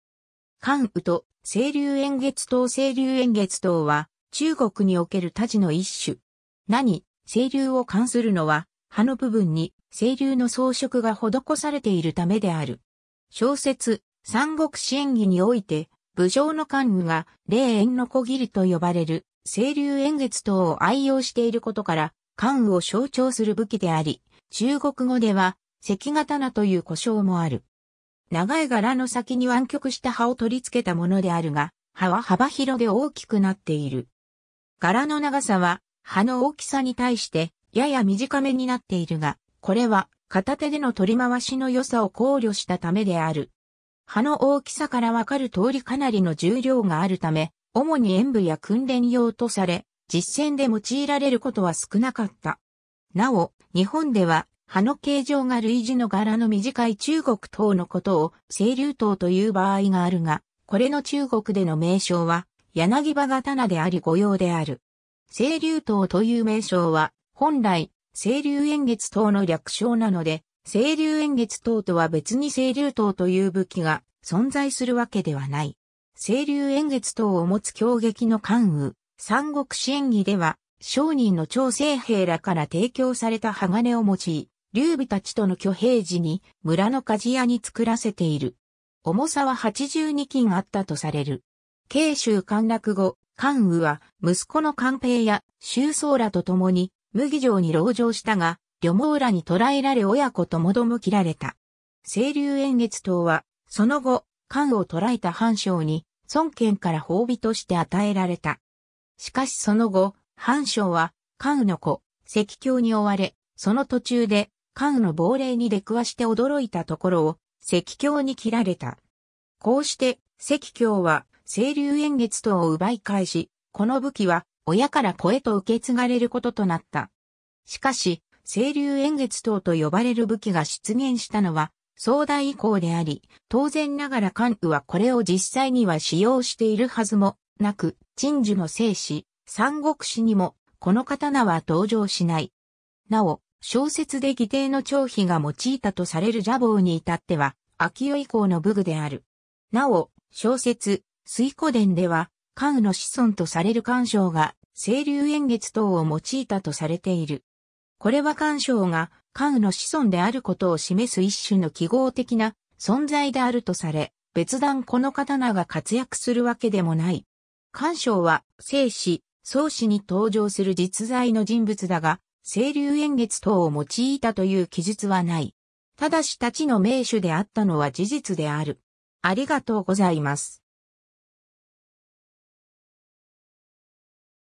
青龍偃月刀
関羽と青龍偃月刀（頤和園・北京） 青龍偃月刀（せいりゅうえんげつとう）は、中国における大刀の一種。
 名に「青龍」を冠するのは、刃の部分に青龍の装飾が施されている為である。
 小説『三国志演義』において、武将の関羽が冷艶鋸（れいえんきょ）と呼ばれる青龍偃月刀を愛用していることから、関羽を象徴する武器であり、中国語では関刀（グアンダオ）という呼称もある。
 長い柄の先に湾曲した刃を取り付けたものであるが、刃は幅広で大きくなっている。
 柄の長さは刃の大きさに対してやや短めになっているが、これは（主に馬上で）片手での取り回しの良さを考慮した為である。
 刃の大きさから分かる通りかなりの重量がある為、主に演舞や訓練用とされ、実戦で用いられることは少なかった。
 なお日本では刃の形状が類似の柄の短い中国刀のことを青龍刀と言う場合があるが、これの中国での名称は柳葉刀であり誤用である。
「青龍刀」という名称は本来「青龍偃月刀」の略称なので、青龍偃月刀とは別に青龍刀という武器が存在するわけではない。
 青龍偃月刀をもつ京劇の関羽（俳優は殷秋瑞） 『三国志演義』では、商人の張世平らから提供された鋼を用い、劉備たちとの挙兵時に村の鍛冶屋に作らせている。
重さは八十二斤あった（後漢時代の度量衡計算では1斤＝222.4gで約18kg、三国志演義が成立した元末 - 明代では50kg弱）とされる。
荊州陥落後、関羽は息子の関平や、周倉らと共に麦城に籠城したが、呂蒙らに捕らえられ親子ともども斬られた。
青龍偃月刀はその後、関羽を捕らえた潘璋に孫権から褒美として与えられた。
しかしその後、潘璋は関羽の子・関興に追われ、その途中で関羽の亡霊に出くわして驚いたところを関興に斬られた。
こうして関興は青龍偃月刀を奪い返し、この武器は親から子へと受け継がれることとなった。
 しかし青龍偃月刀と呼ばれる武器が出現したのは宋代以降であり、当然ながら関羽はこれを実際には使用しているはずもなく、陳寿の正史『三国志』にもこの刀は登場しない。
なお、小説で義弟の張飛が用いたとされる蛇矛にいたっては明代以降の武具である。
 なお、小説『水滸伝』では関羽の子孫とされる関勝が青龍偃月刀を用いたとされている。
これは関勝が関羽の子孫であることを示す一種の記号的な存在であるとされ、別段この刀が活躍するわけでもない。
関勝は正史『宋史』に登場する実在の人物だが、青龍偃月刀を用いたという記述はない。
ただし大刀の名手であったのは事実である。